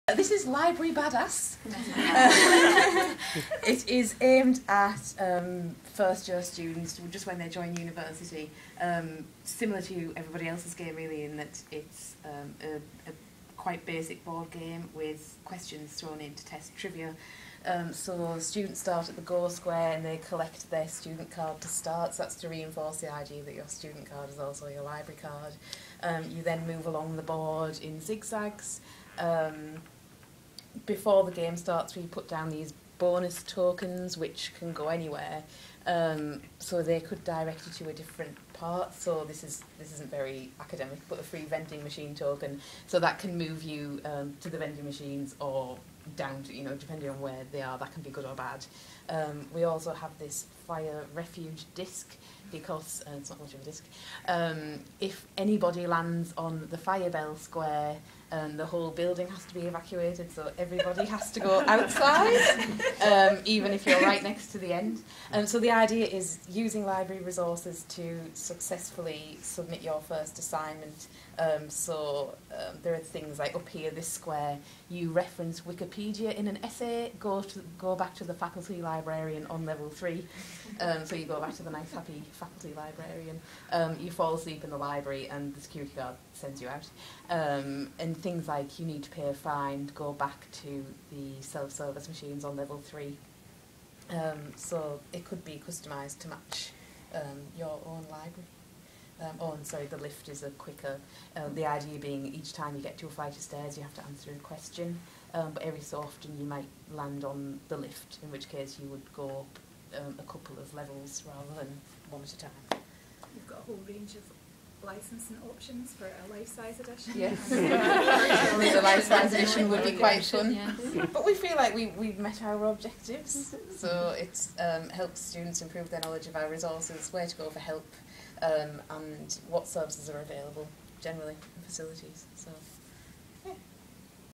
Library BA-dass
Prototype game from SALCTG games for libraries session in Glasgow, 2015.